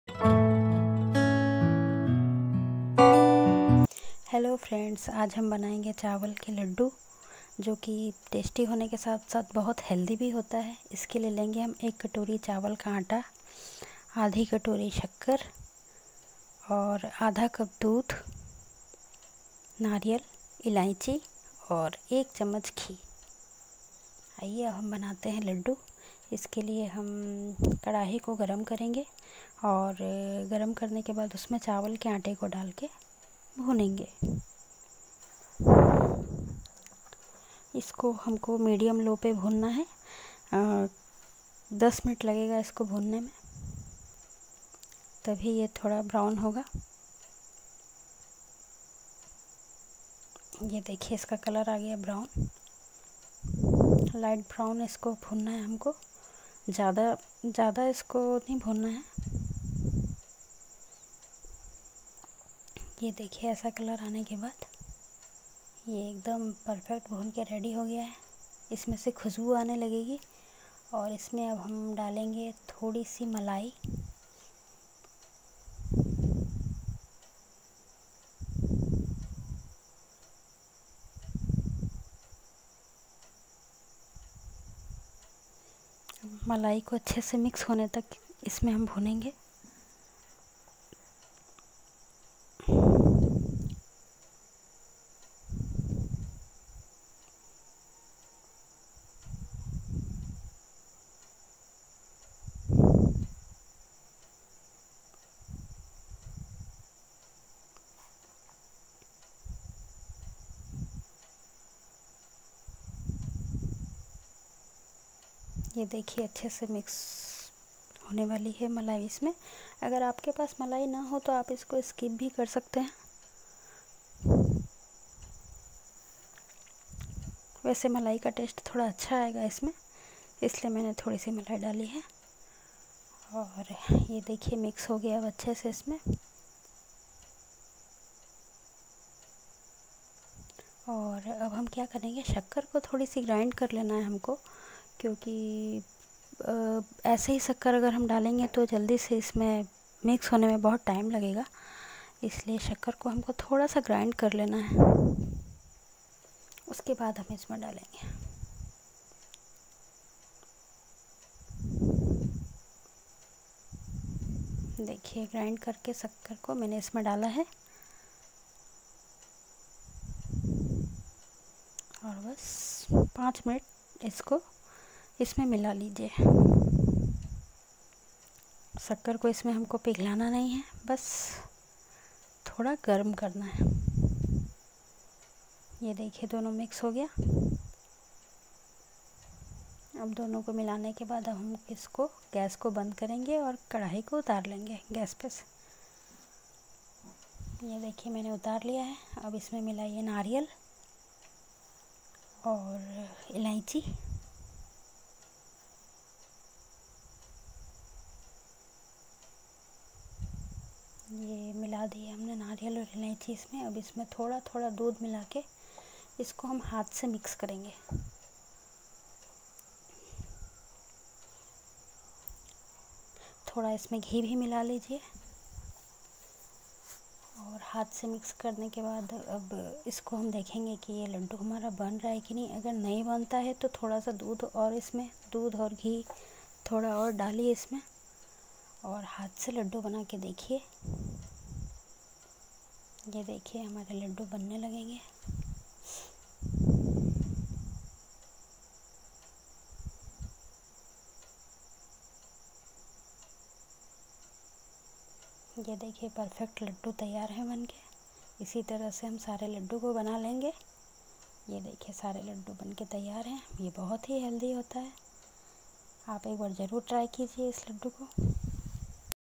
चावल के लड्डू / rice laddu recipe
Is vedio me chawal k aate se laddu banaye gye h...
Jo ki bahut hi tasty and healthy h